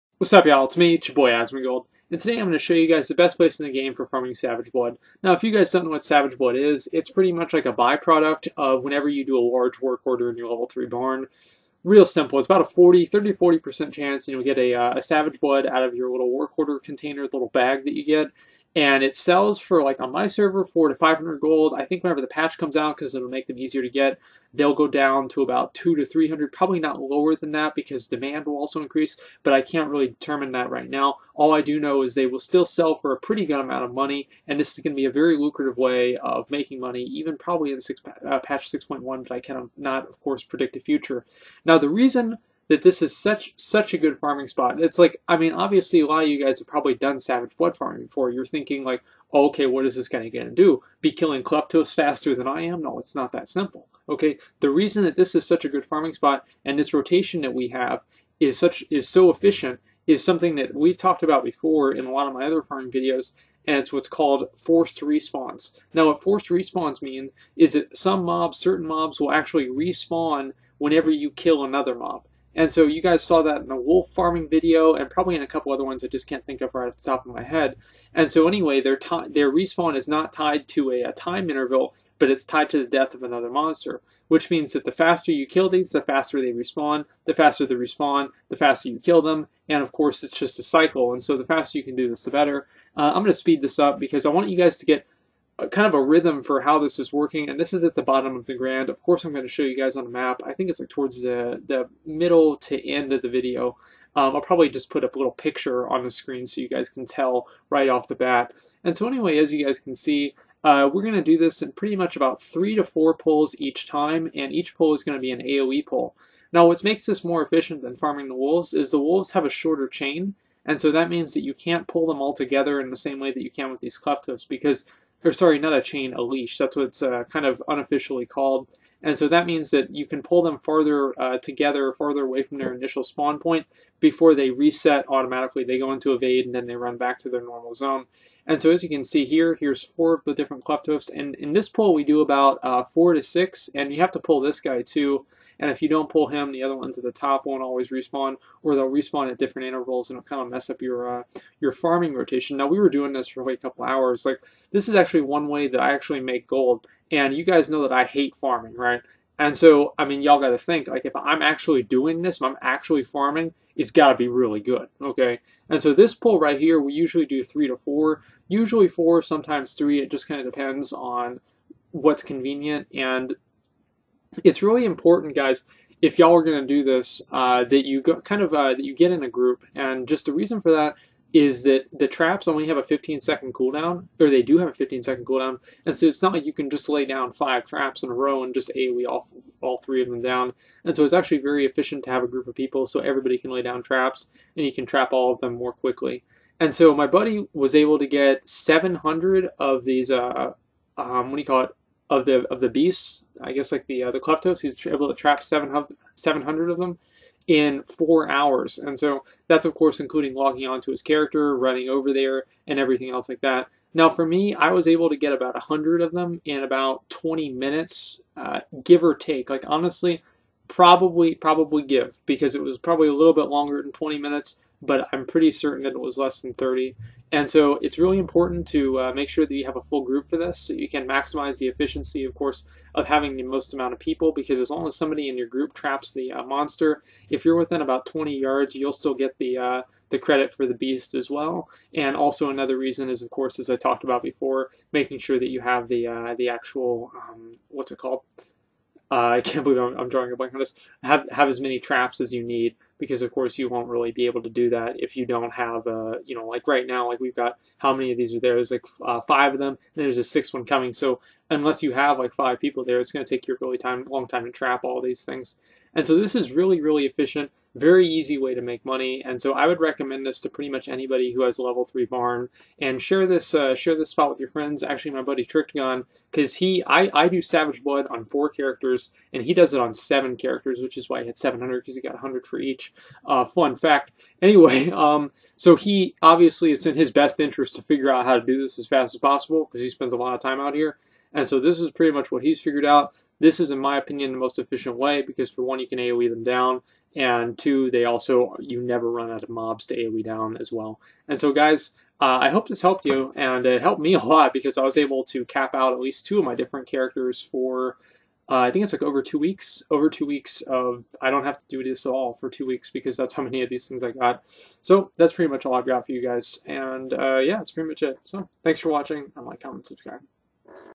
The Best Place to Farm Savage Blood-Forced Respawn Clefthoof Farming
Title says it all, I'm not really sure how many you can get per hour, but I know I got over 100 in less than a half hour, so it's a lot!